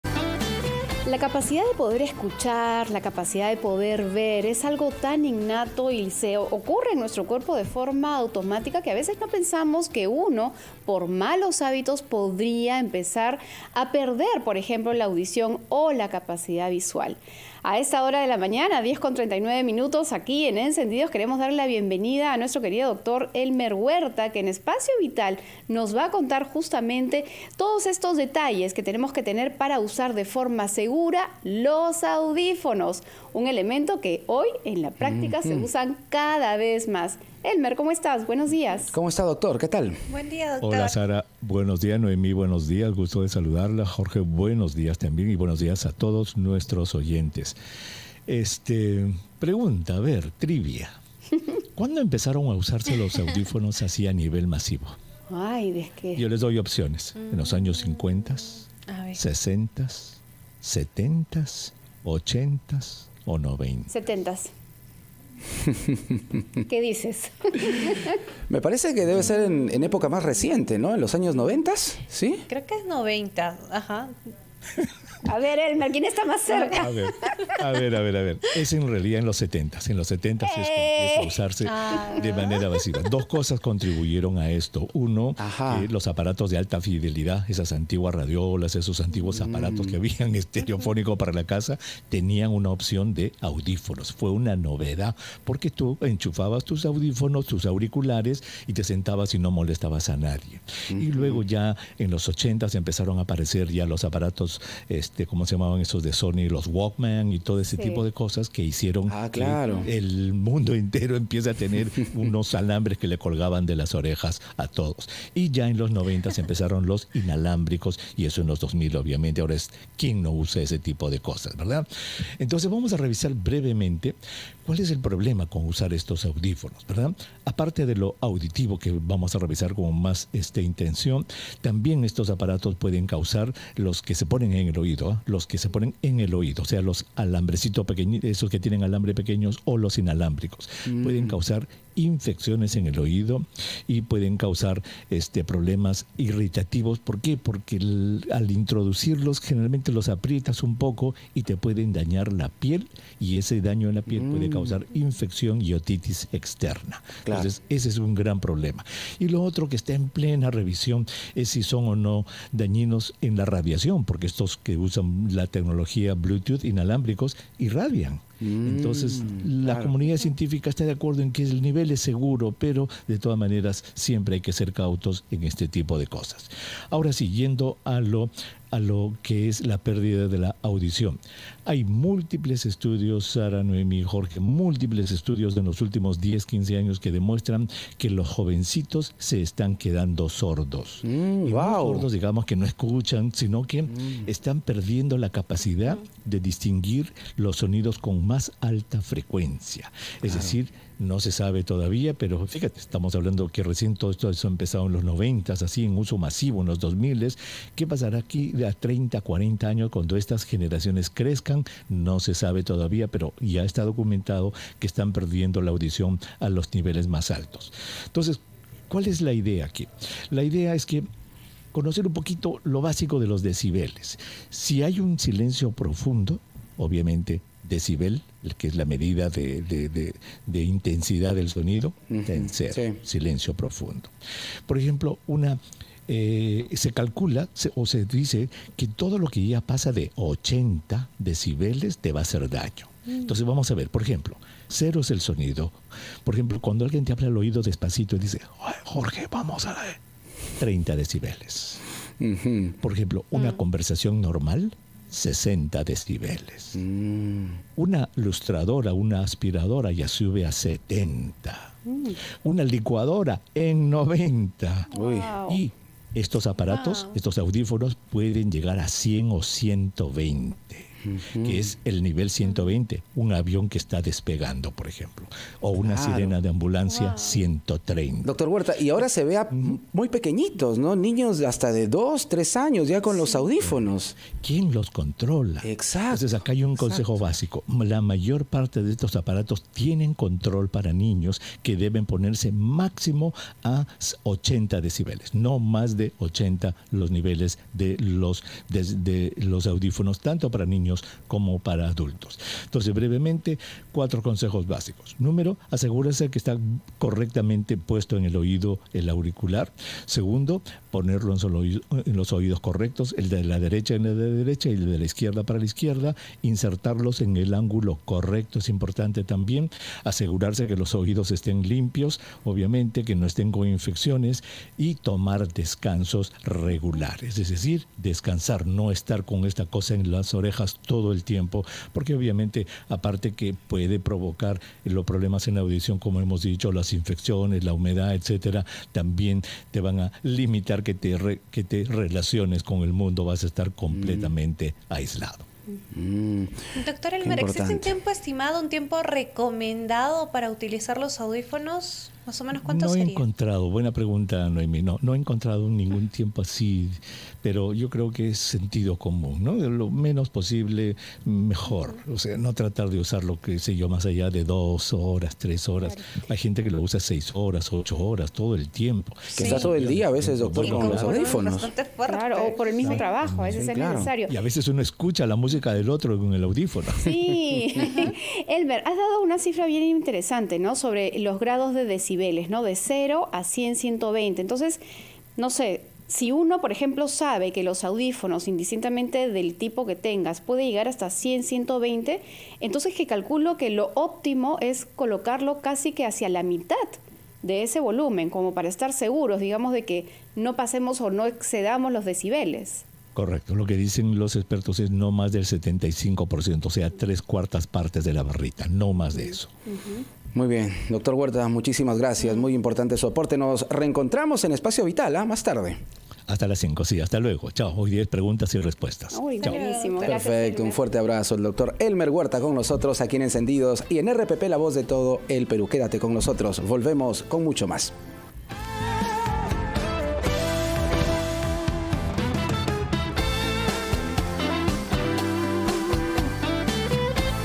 Pérdida de la audición: ¿Cuál es la manera más segura de usar los audífonos?
Nuestros oídos son una de nuestras ventanas al mundo que nos rodea y por eso es importante que nos preocupemos por ellos.